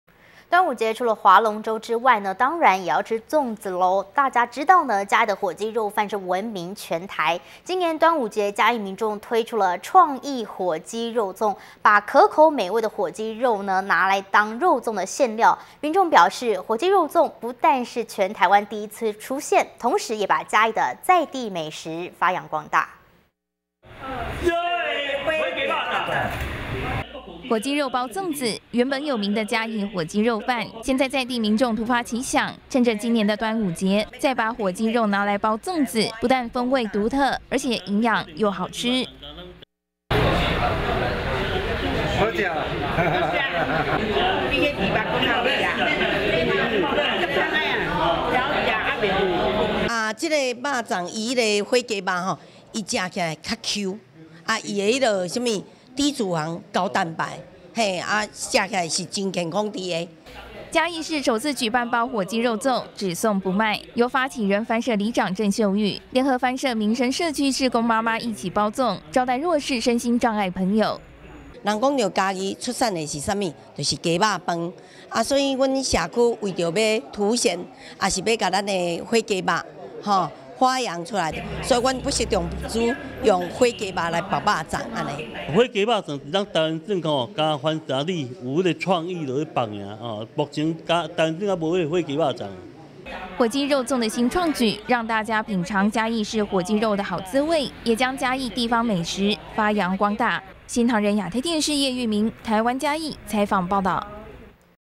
【美食新聞】嘉義火雞肉粽 新創小吃發揚在地美味
(NTDAPTV) 嘉義的火雞肉飯聞名全台，今年端午節嘉義民眾，推出創意火雞肉粽， 把可口美味的火雞肉，拿來當肉粽餡，民眾表示「火雞肉粽」不但是台灣首次出現，同時也把嘉義的在地美食發揚光大。【新唐人亞太台2013年6月9日訊】

「熱ㄟ的火雞肉粽！」

火雞肉包粽子，原本有名的嘉義火雞肉飯，現在在地民眾突發奇想 ， 趁著今年的端午節，再把火雞肉拿來包粽子， 不但風味獨特，而且營養又好吃。

社區住民：「好吃，比豬肉還好吃，很香、很好吃，也不油膩。」

嘉義市番社里里長 鄭秀玉：「這個肉粽它用火雞肉，吃起來較Q，它是低脂肪高蛋白，吃起來很健康。」

嘉義市首次舉辦包火雞肉粽，只送不賣，由發起人番社里長鄭秀玉，聯合番社民生社區志工媽媽一起包粽，招待弱勢身心障礙朋友。

嘉義市番社里里長 鄭秀玉：「一說到嘉義出產是什麼?就是雞肉飯，所以我們社區為了要凸顯，把我們的火雞肉發揚出來，所以我們不惜重資，用火雞肉來包肉粽。」

嘉義市番社民生社區發展協會理事長 蔡坤章：「火雞肉粽，咱們臺灣只有番社里有這個創意去包，目前臺灣全省還沒有這種火雞肉粽。」

火雞肉粽的新創舉，讓大家品嘗嘉義市火雞肉的好滋味，也將嘉義地方美食發揚光大。